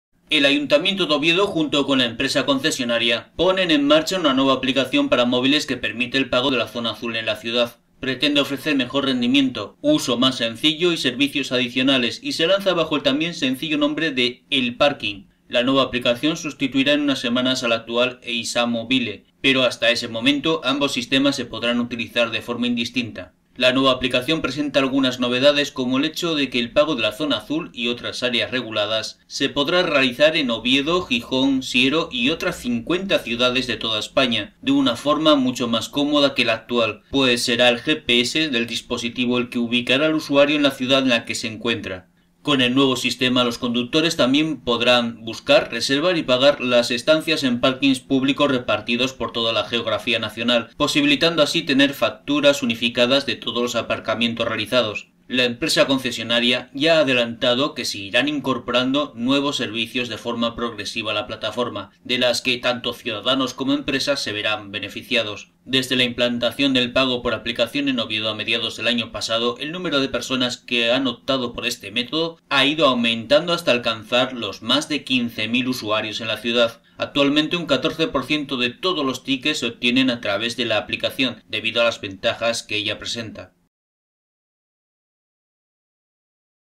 Ayuntamiento de Oviedo pone en marcha ElParking una nueva APP para mejorar el pago en la zona azul
AsturiasVerde.Net NOTICIA: El Ayuntamiento de Oviedo, junto con la empresa concesionaria, ponen en marcha una nueva aplicación para móviles que permite el pago de la zona azul en la ciudad.
Pretende ofrecer mejor rendimiento, uso más sencillo y servicios adicionales, y se lanza bajo el también sencillo nombre de ElParking. La nueva aplicación sustituirá en unas semanas a la actual Eysamo-bile, pero hasta ese momento ambos sistemas se podrán utilizar de forma indistinta.
La nueva app presenta algunas novedades como el hecho de que el pago de la zona azul y otras áreas reguladas se puede realizar en Oviedo, Gijón, Siero, y otras 50 ciudades de toda España de un forma mucho más cómoda que en la actual, pues es el GPS del dispositivo el que ubica al usuario en la ciudad en la que se encuentra. Con el nuevo sistema, los conductores también pueden buscar, reservar y pagar las estancias en parkings públicos repartidos por toda la geografía nacional, posibilitando así tener facturas unificadas de todos los aparcamientos realizados. La empresa concesionaria ya ha adelantado que se irán incorporando nuevos servicios de forma progresiva a la plataforma de las que tanto ciudadanos como empresas se verán beneficiados.
Desde la implantación del pago por app en Oviedo a mediados del año pasado el número de personas que opta por este método ha ido aumentando hasta alcanzar los más de 15.000 usuarios en nuestra ciudad. Actualmente un 14% de todos los tickets se obtienen a través de la app debido a las ventajas que presenta.
-Fuente: Nota de prensa del Ayuntamiento de Oviedo.-